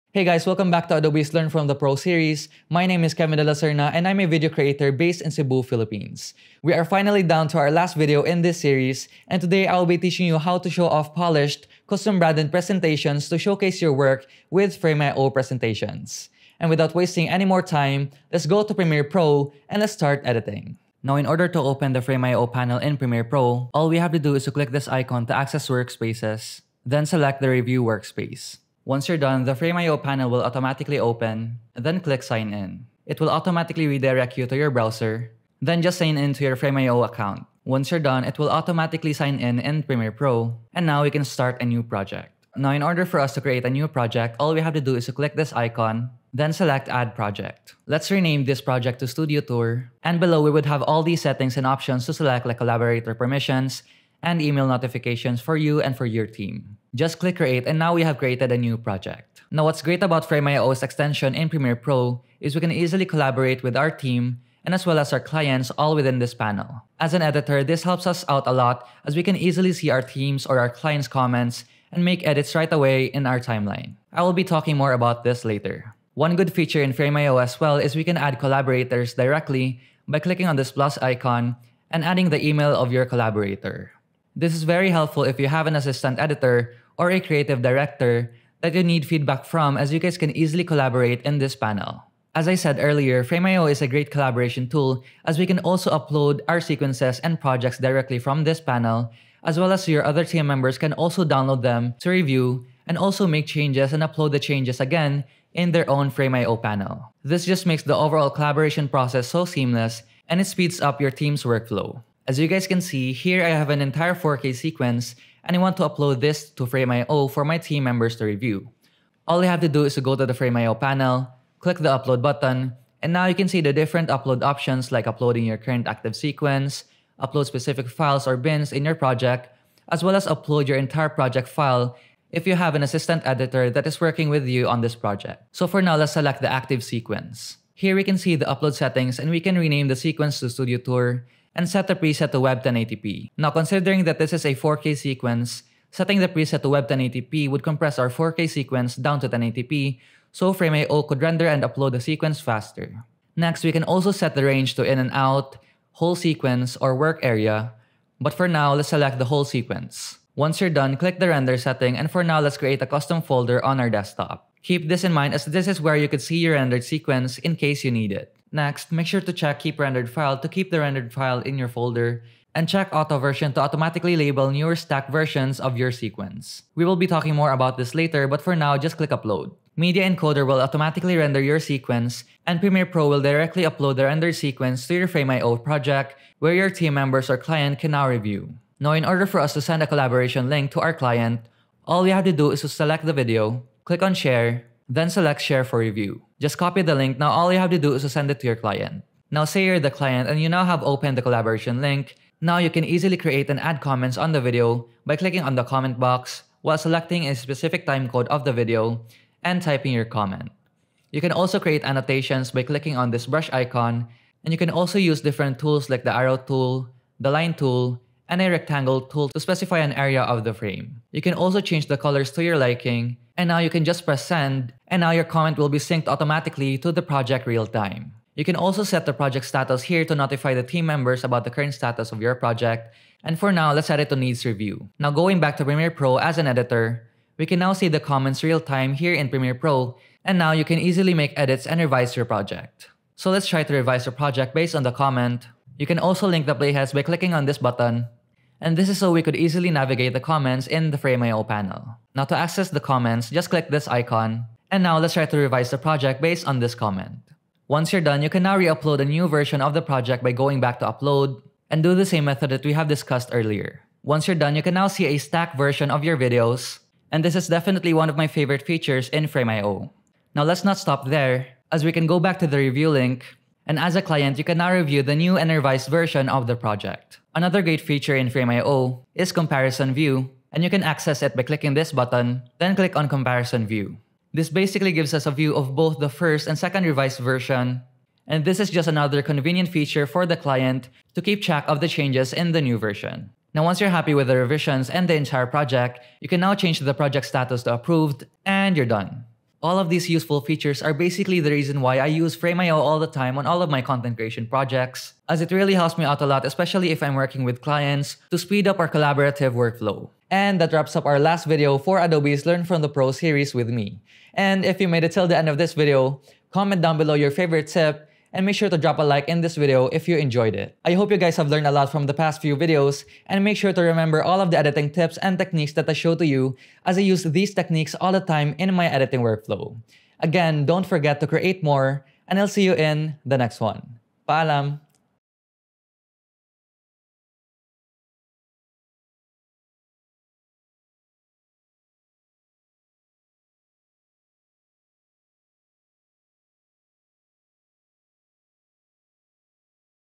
Creating Custom-Branded Presentations in Frame.io | Learn from the Pros
In this video from the Learn from the Pros Series, learn how video creator Kevin De La Cerna collaborates with different teams, showing his work from to start and finish, using Frame.io Presentations on PremierePro.

CHAPTERS
00:00 - 00:16 - Introduction
00:17 - 01:04 - Accessing Frame.io in Premiere Pro an Creating a Project
01:05 - 01:52 - Collaboration
10:53 - 03:11 - Uploading Sequence
03:12 -04:03 - Sharing Collab Link & Adding Comments
04:04 -04:59 - Revisions
05:00 - 05:50 - Outro

Creating Custom-Branded Presentations in Frame.io | Learn from the Pros | Adobe Video